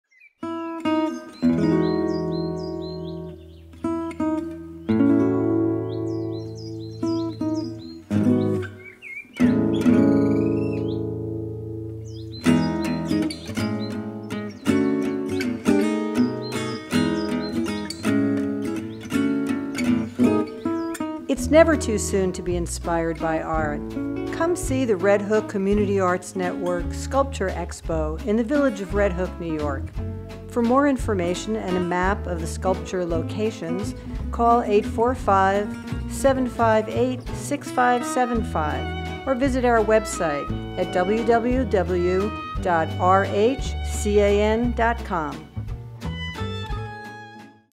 Red Hook Community Arts Network Sculpture Expo PSA: "Inspired"
It’s never too soon to be inspired by art.  Come see the Red Hook Community Arts Network Sculpture Expo in the Village of Red Hook, NY.